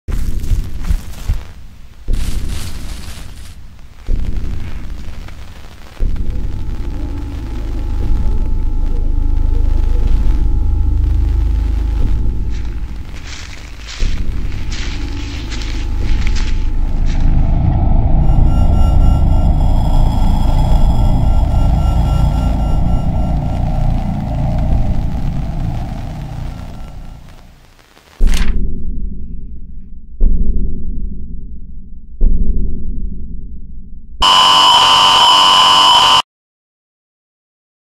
Slender Odenwald - Teaser
Der Teaser zu unserem geplanten Film "Slender Odenwald", der, wie der Name schon verrät eine Realverfilmung des bekannten Horror-Survivalspiels "Slenderman", ist. Das geplante Ergebnis soll in Spielfilmlänge erscheinen.

Der Film wird keine 0815-Version einer Verfilmung, sondern wir investieren viel Zeit, Geld und Herzblut in das Projekt und freuen uns jetzt schon auf die Premiere.

Abonieren für mehr Infos :)